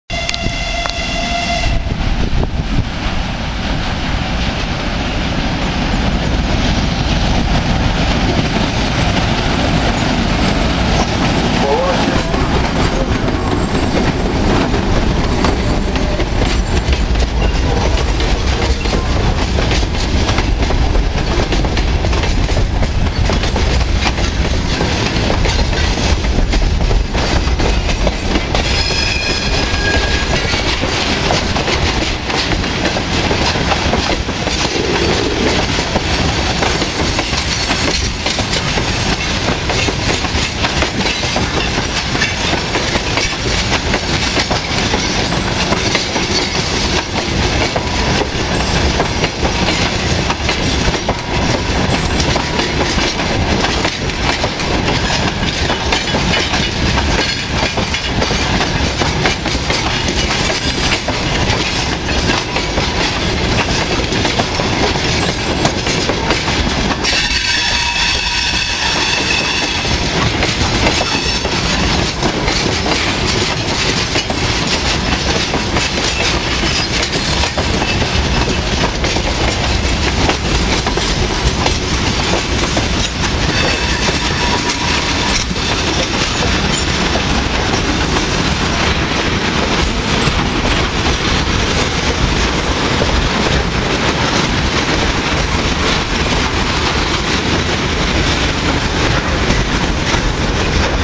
2М62-0909 с грузовым составом.avi
2М62-0909 с грузовым составом прибывает на ст.Коломыя.